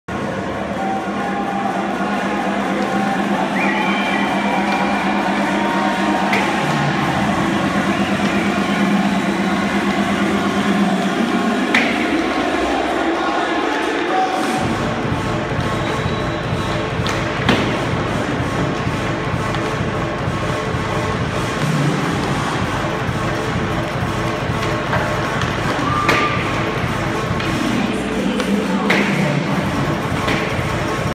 2017 Memorial Cup: Sea Dogs/Otters take the ice for warmup
The Saint John Sea Dogs and Erie Otters take the ice for warmup at the 2017 Memorial Cup at the WFCU Centre in Windsor, Ontario on May 26, 2017.